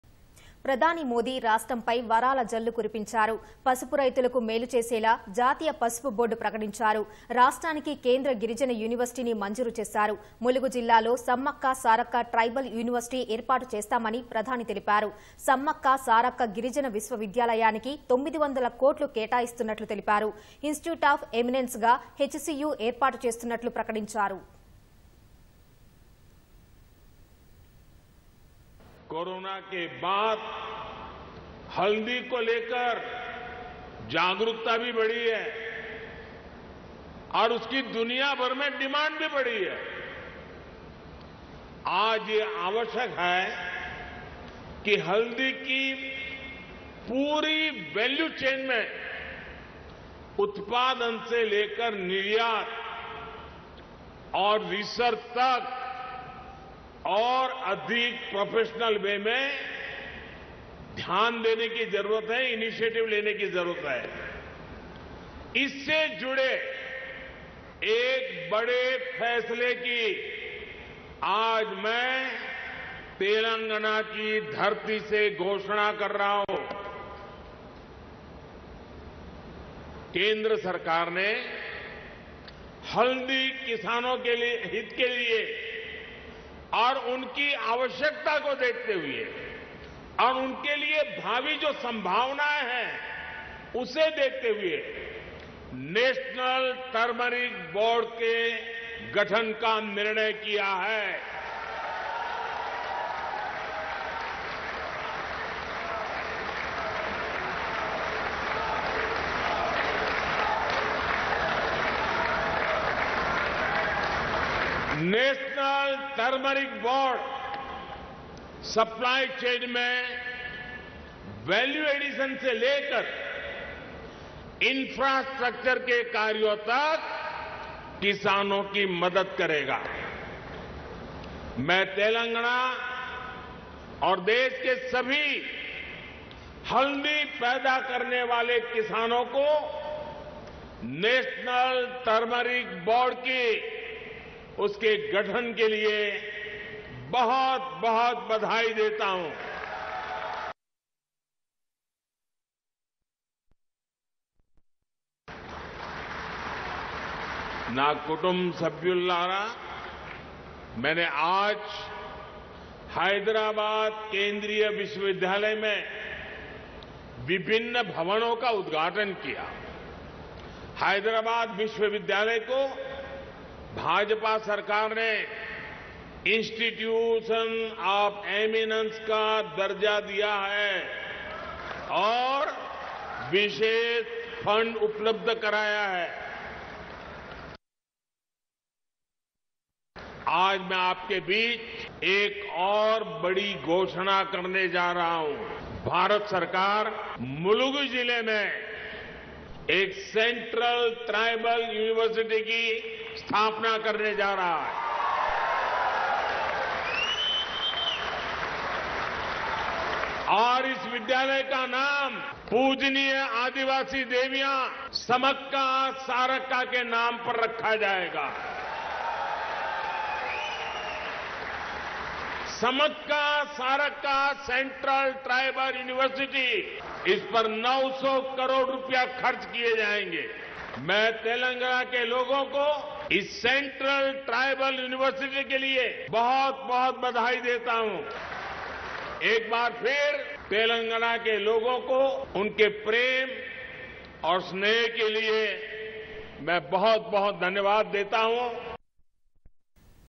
Formation Of National Turmeric Board To TS | PM Modi Announced | తెలంగాణ రాష్ట్రానికి పసుపు బోర్డు
ప్రధాని మోదీ రాష్ట్రంపై వరాల జల్లు కురిపించారు. పసుపు రైతులకు మేలు చేసేలా జాతీయ పసుపు బోర్డు ప్రకటించారు. రాష్ట్రానికి కేంద్ర గిరిజన యూనివర్సిటీని మంజూరు చేశారు. ములుగు జిల్లాలో సమ్మక్క-సారక్క ట్రైబల్ యూనివర్సిటీ ఏర్పాటు చేస్తామని ప్రధాని తెలిపారు.   సమ్మక్క- సారక్క గిరిజిన విశ్వవిద్యాలయానికి  900 కోట్లు కేటాయిస్తున్నట్లు తెలిపారు.  ఇన్ స్టిట్యూట్  ఆఫ్  ఎమినెన్స్ గా  హెచ్ సీయూ ఏర్పాటు చేస్తున్నట్లు ప్రకటించారు